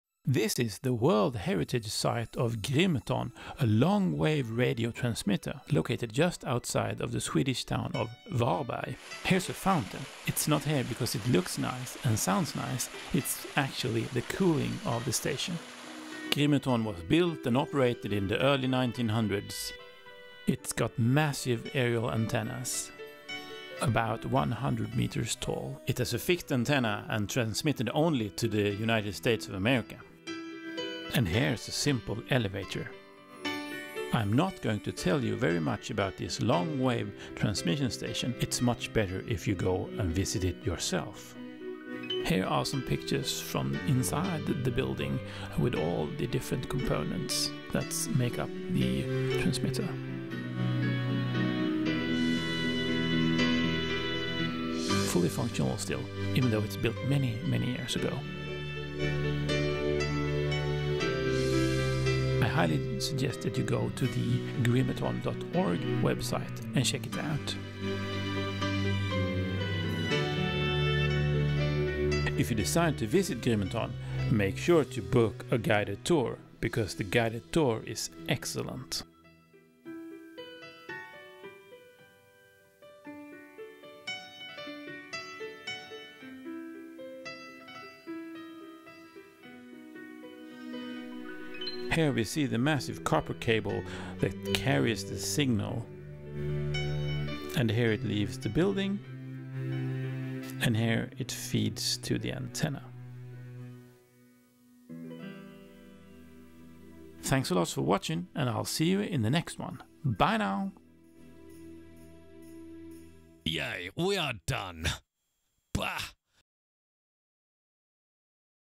From Sweden to the USA: The Grimeton Story!
Embark on a historical journey from Sweden to the USA, told through the fascinating story of the Grimeton Transatlantic Radio Transmitter! Nestled just outside the quaint town of Varberg, this World Heritage Site holds secrets and stories of early 20th-century wireless communication. This video showcases the awe-inspiring aerial antennas that rise 100 meters tall, the cooling fountain integral to the station's operation, and the intricate interior of the transmitter, still fully functional today. I've had the privilege of exploring this monumental site with my daughter in 2018 and can't wait to share our experience with you. If this piques your interest, I urge you to visit Grimeton.org and better yet, book a guided tour for an immersive experience. Here's a taste of what you can expect on such an adventure.

NERD INFORMATION
Camera:Panasonic G80
Lens:Do not remember but probably Lumix 14-140mm
Sound:In camera with video mic pro from Röde but sound only used in the fountain scene becuse it was raining an my daughter was far from quiet : )
Video editor: Davinci Resolve 16.2 studio

No:0082